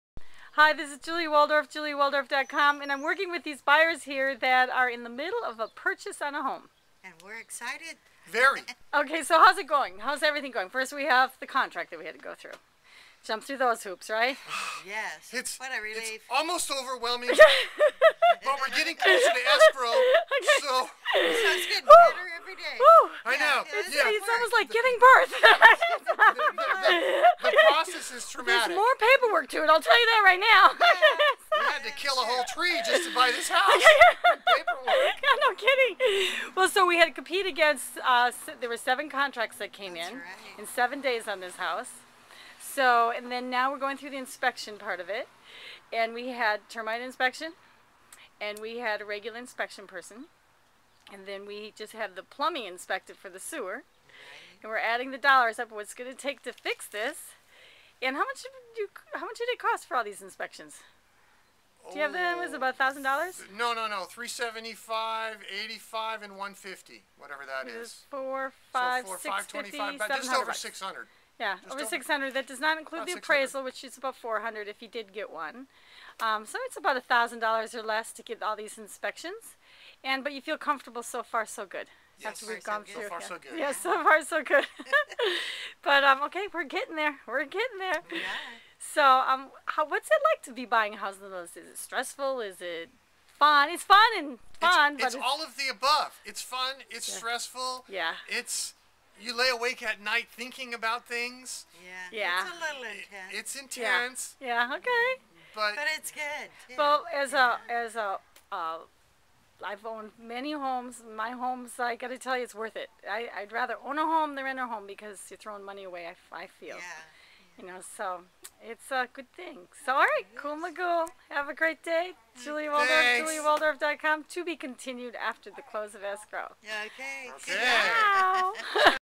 Home Buyers in the Process!
interviews the home buyers that she is working with.   They explain how it feels and what the costs are through the process. See more Central Coast California real estate information, homeless, single parents, and fun in the sun